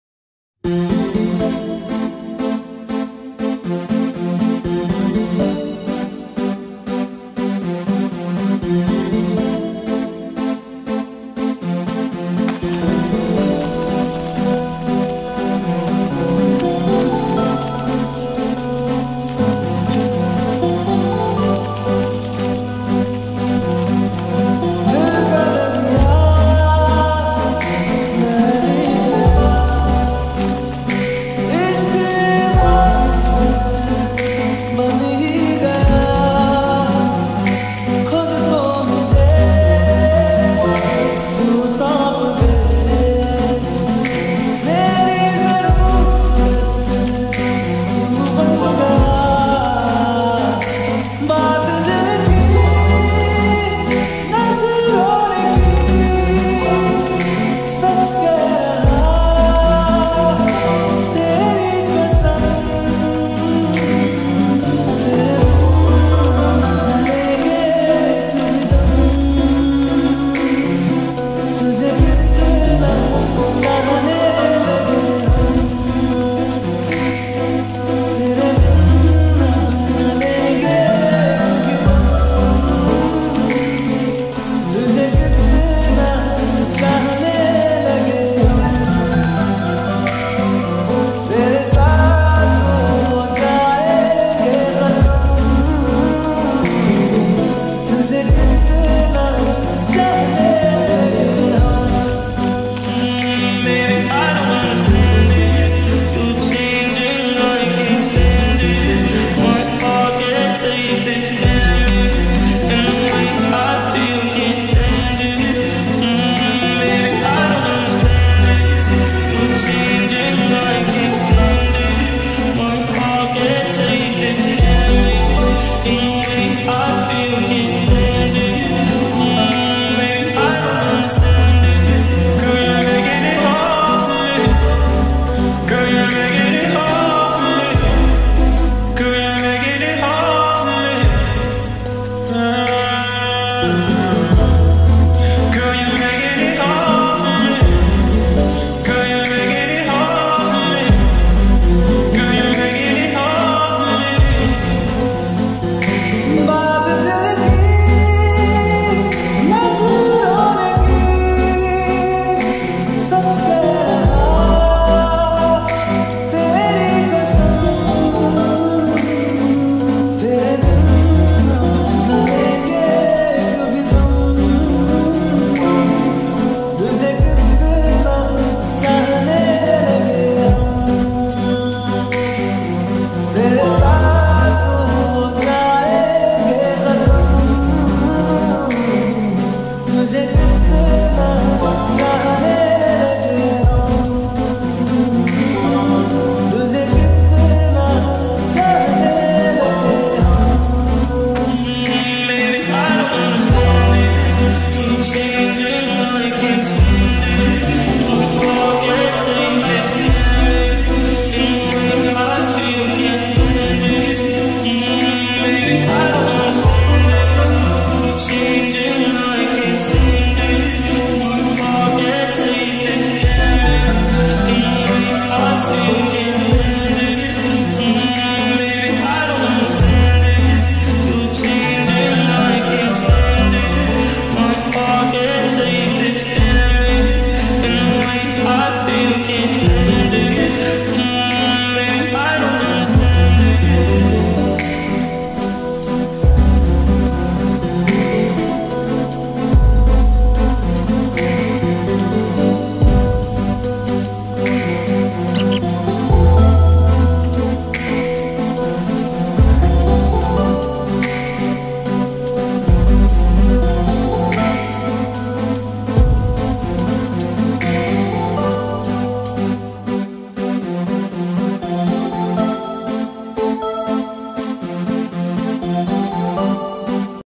I have painted a village scenery using watercolor
watercolor painting step by step tutorial

used brush- mop brush (₹1059)
used paper- chitrapat (₹20)
used colour- watercolor